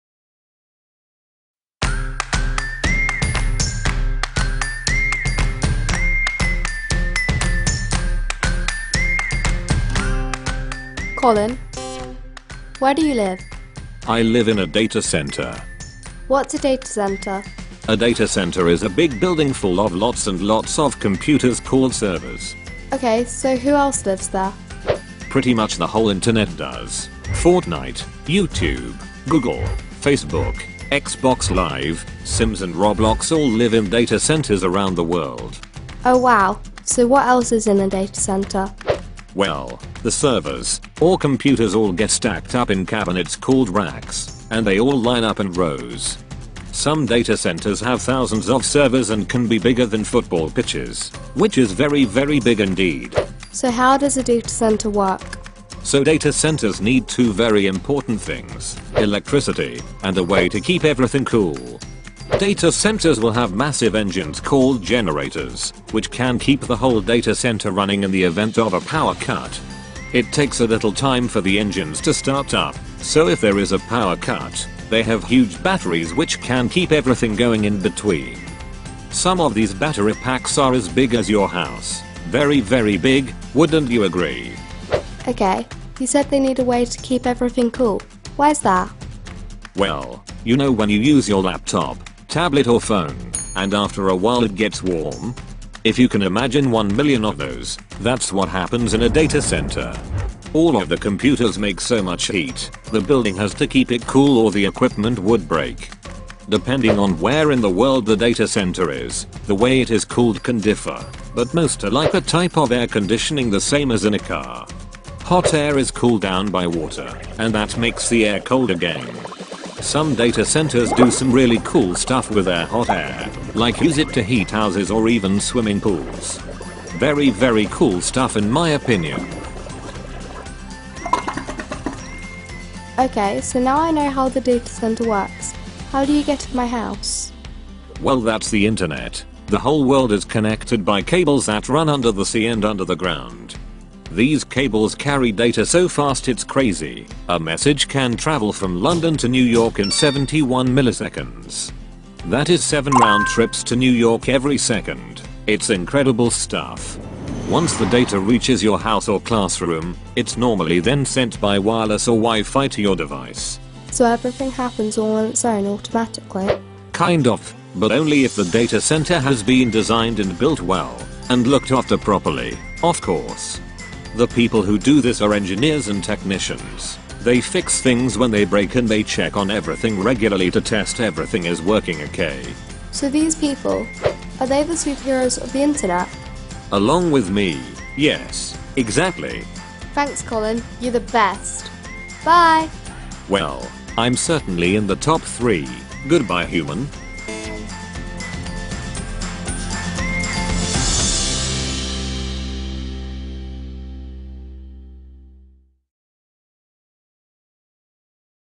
What is a Datacenter?
A short video explaining what a datacenter is, how it works and what they do for society today.